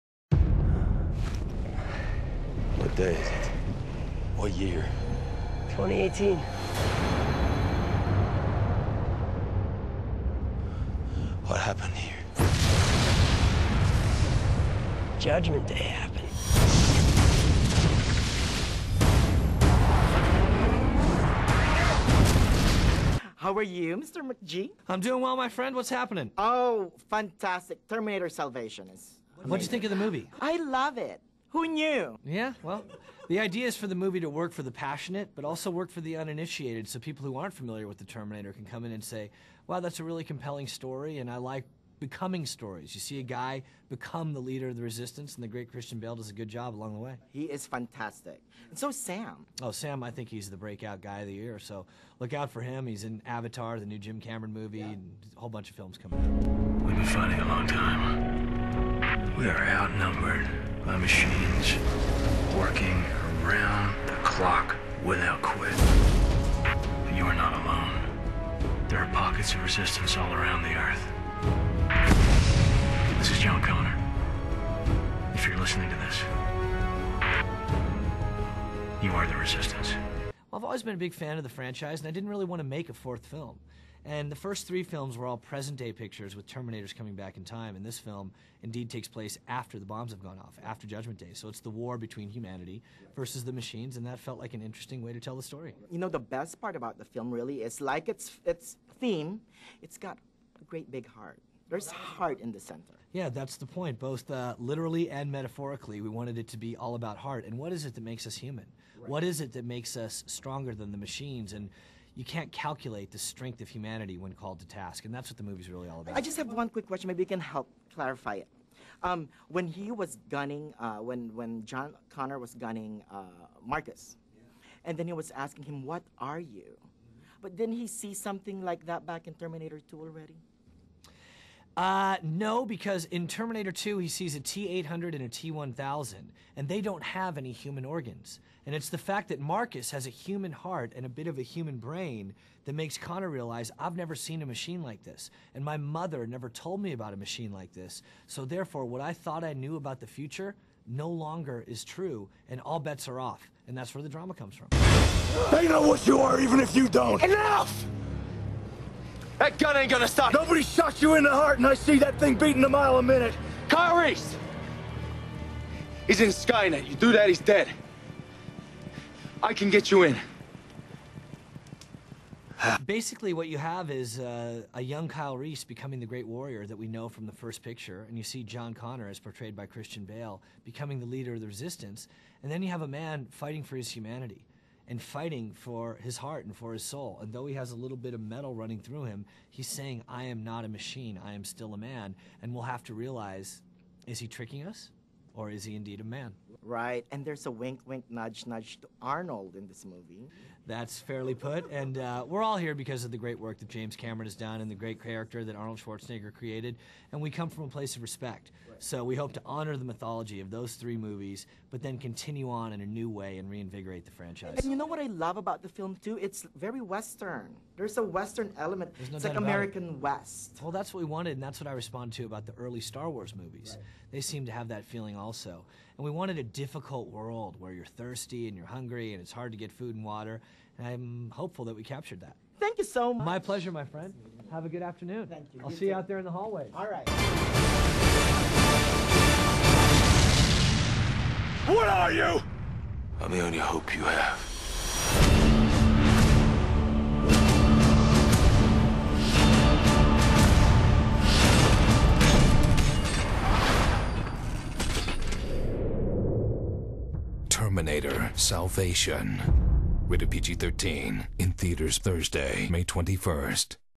McG Interview for Terminator Salvation (Director)
My fun interview with McG, director of "Terminator Salvation."

We talked about:

*** Why make another "Terminator" film?
*** The western feel and tone of "Terminator Salvation"
*** Working with Christian Bale
*** One confusing scene that he helped to enlighten me